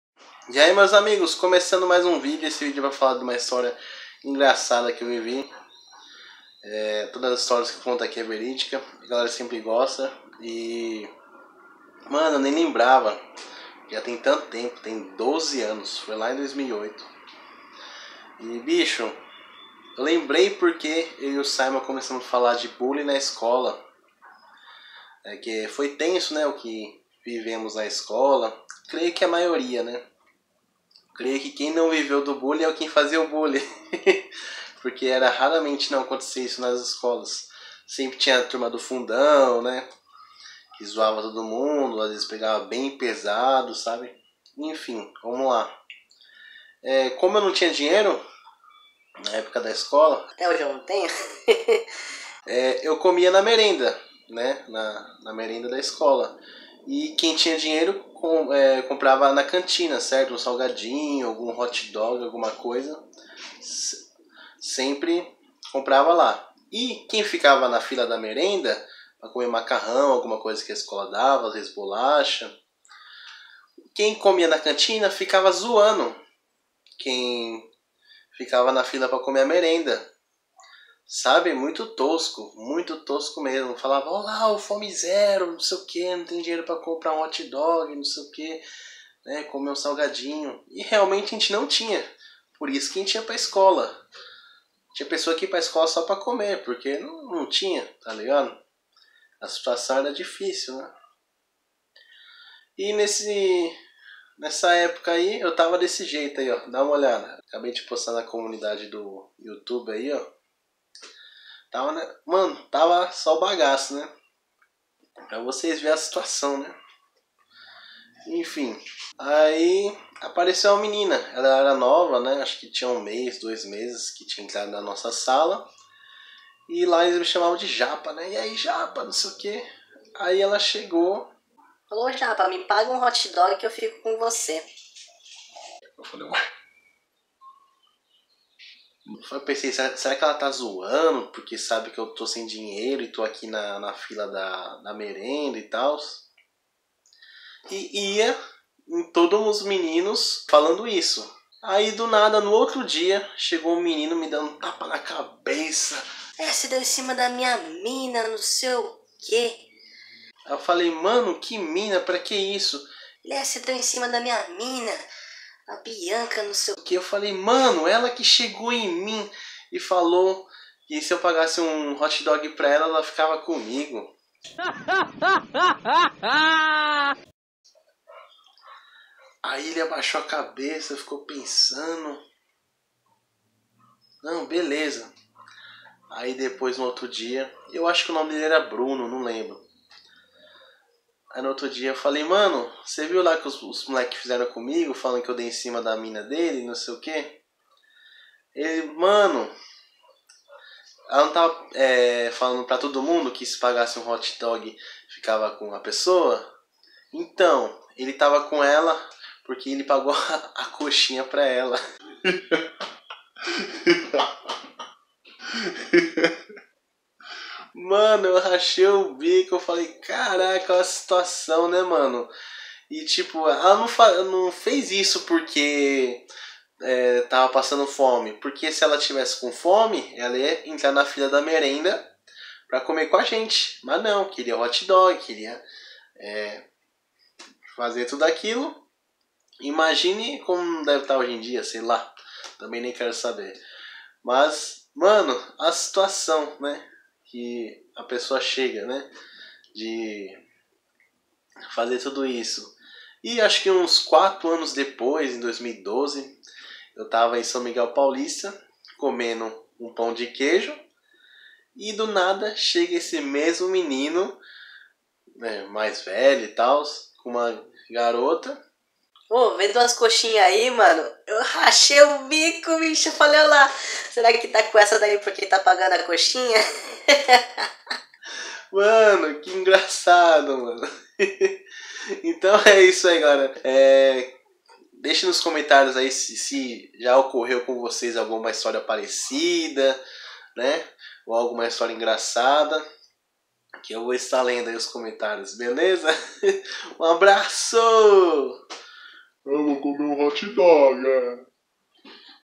A Mina do Hot Dog
e ai pessoal contando mais uma história que vivi
dessa vez essa cilada, mas muito engraçado